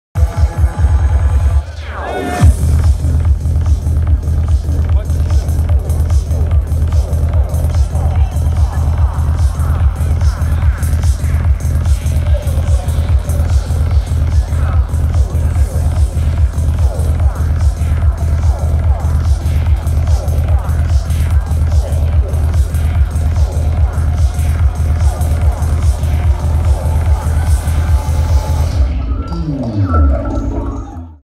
Boom 2016
Having fun with an umbrella during Ajja's set at Boom 2016. If I remember correctly a kind hearted soul who had a shop graciously gifted me this perfect accoutrement for dealing with the blazing sun. I do love the Boom Festival but sometimes the sun can be downright brutal!